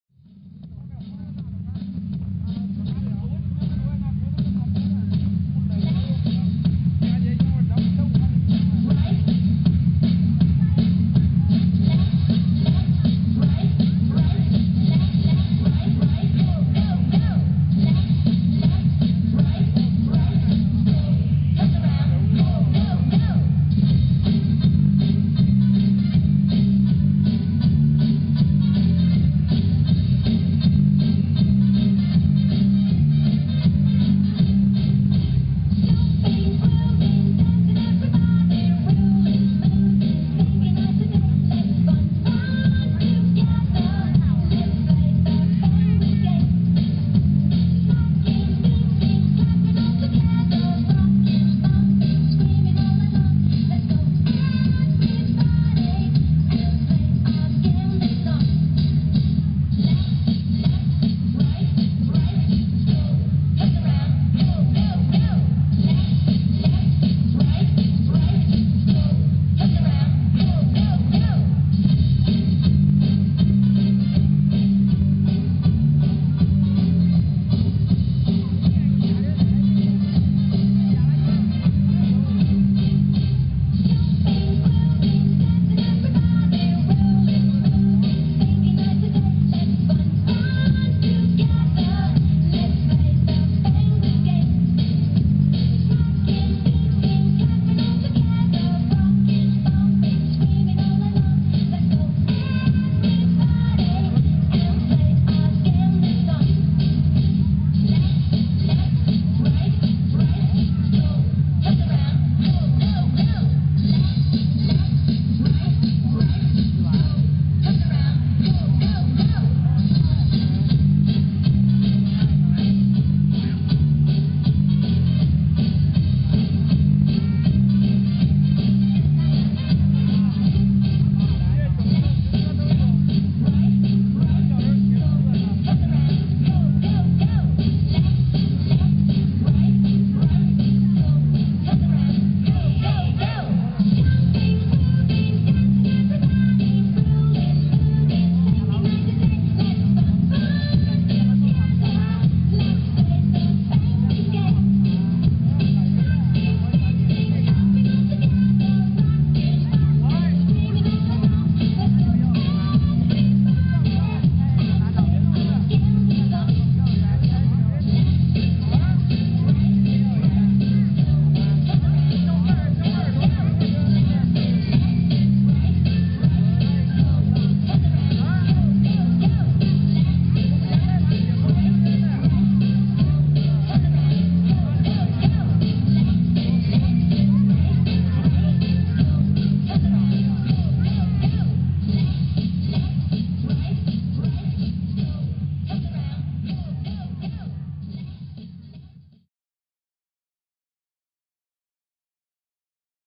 Kindergarten Dance Performance Nanning China
Kindergarten schools usually have a year-end performance for parents and others. This one was on the concourse of my apartment complex in Nanning. Enjoy!